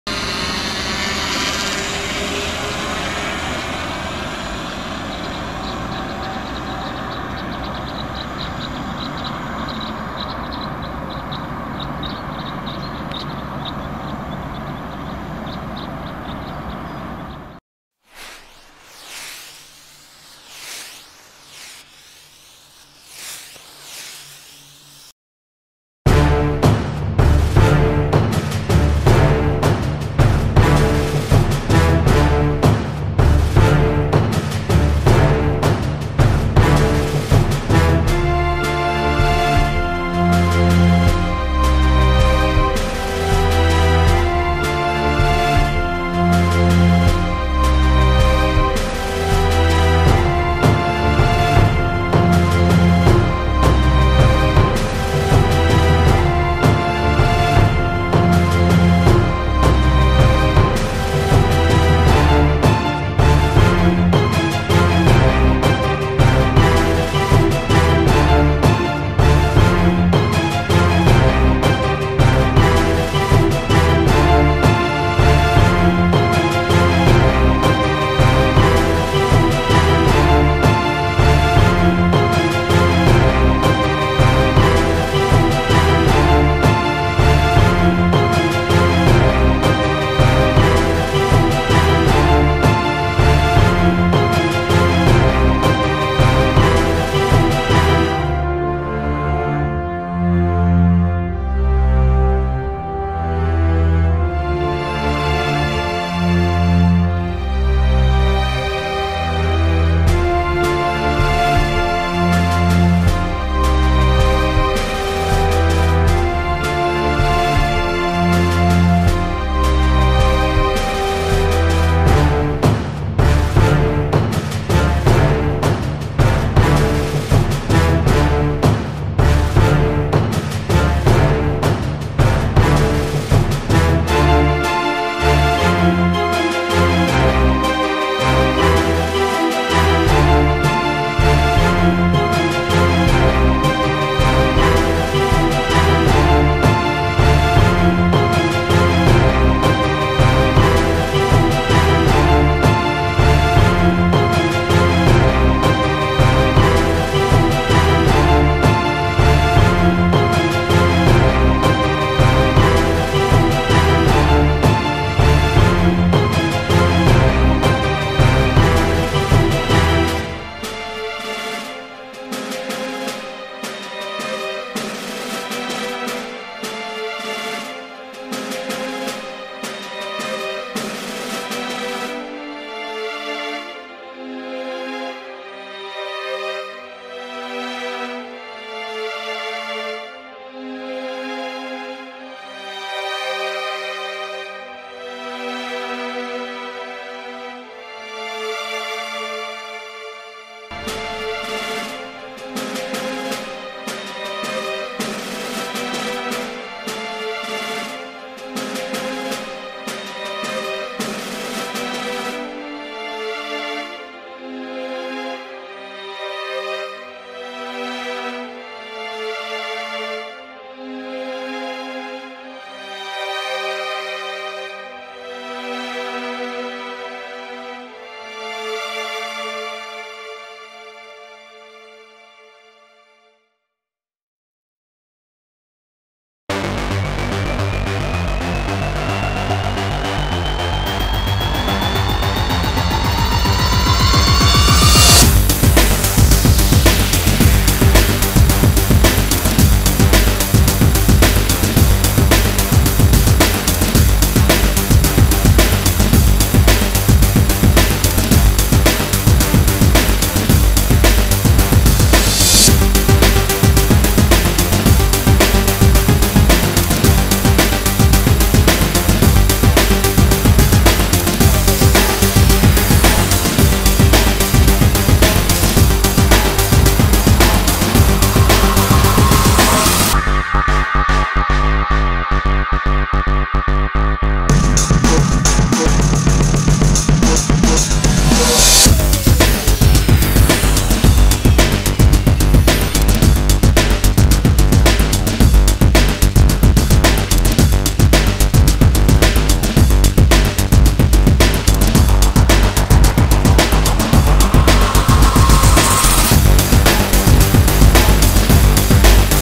Epic Fall Flights - Autumn in Austria - Cinematic drone footage - DJI Mavic Pro
Flying my DJI Mavic Pro drone above the clouds in Austrian autumn nature landscape over lakes, fields and woods. Enjoy the colors of fall.
Aerial footage of could flying without any sport mode + or hacking.

BR Drone Chris